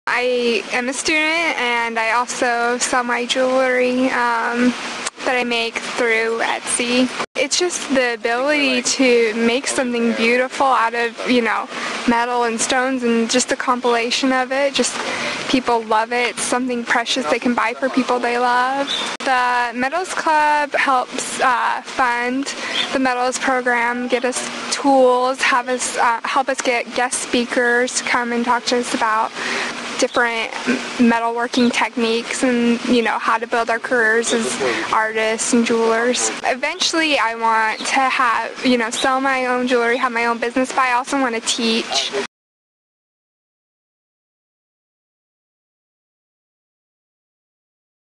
ASU Student Jewelry Maker
Devlyn Kanaga, a senior majoring in metals, was selling jewelry made by herself and other metals students to raise funds for the Metals Club at ASU. Kanaga took a class in high school that inspired her to major in metals. She crafted several pieces for sale on the table in time for Valentine's Day. In the future she hopes to continue making jewelry and teach.  Kanaga discusses her passion for creating jewelry, the Metals Club, and her career goals in an audiovisual slide show. This story was created by Producer/Editor/Reporter: Laura Palmisano